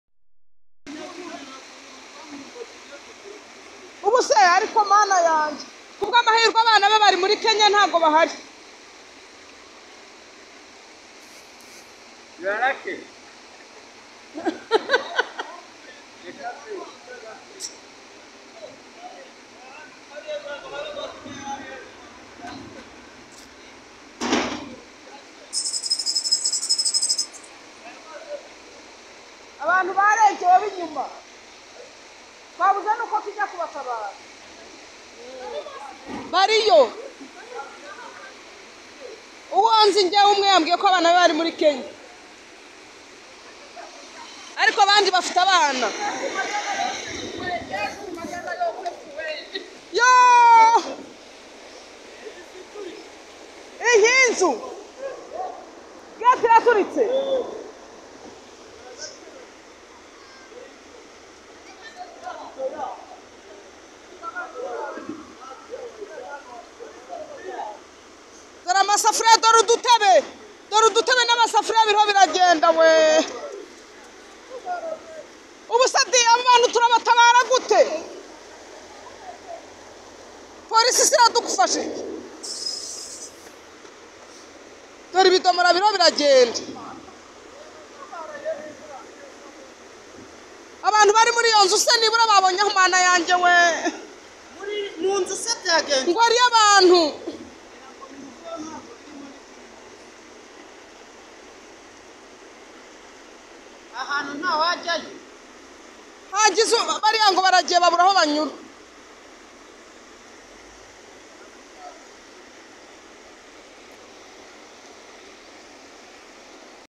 Imvura Idasanzwe yaguye yasenye ibintu byinshi part1
Imvura Idasanzwe yaguye yasenye ibintu byinshi mu mujyi wa Kigali.